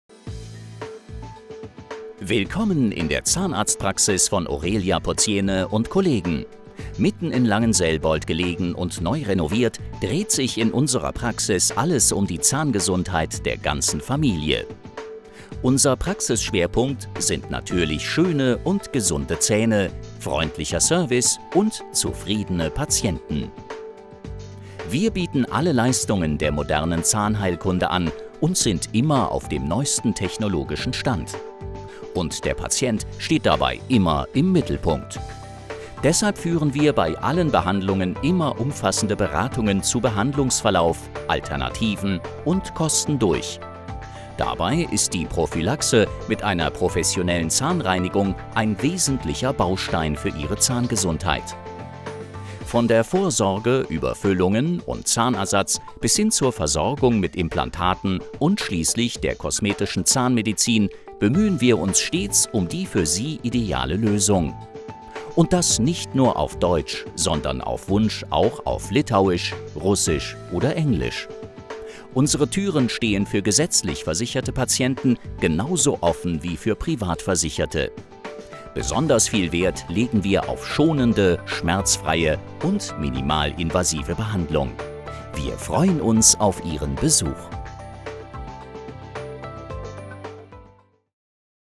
Zahnarztpraxis Aurelija Pociene & Kollegen| Unternehmensfilm
Willkommen in der Zahnarztpraxis von Aurelija Pociene und Kollegen!
Mitten in Langenselbold gelegen und neu renoviert, dreht sich in ihrer Praxis alles um die Zahngesundheit der ganzen Familie. Ihr Praxisschwerpunkt sind natürlich schöne und gesunde Zähne, freundlicher Service und zufriedene Patienten.
Sie bieten alle Leistungen der modernen Zahnheilkunde an und sind immer auf dem neuesten technologischen Stand. Und der Patient steht dabei immer im Mittelpunkt. Deshalb führen sie bei allen Behandlungen immer umfassende Beratungen zu Behandlungsverlauf, Alternativen und Kosten durch. Dabei ist die Prophylaxe mit einer professionellen Zahnreinigung  ein wesentlicher Baustein für Ihre Zahngesundheit. - Von der Vorsorge über  Füllungen und Zahnersatz bis hin zur Versorgung mit Implantaten und schließlich der  kosmetischer Zahnmedizin  bemühen sie sich stets um die für Sie ideale Lösung. Und das nicht nur auf Deutsch, sondern auf Wunsch auch auf litauisch, russisch oder englisch. Ihre Türen stehen für gesetzlich versicherte Patienten genauso offen, wie für privat Versicherte. Besonders viel Wert legen sie auf schonende, schmerzfreie und minimalinvasive Behandlung. Sie freuen sich auf Ihren Besuch!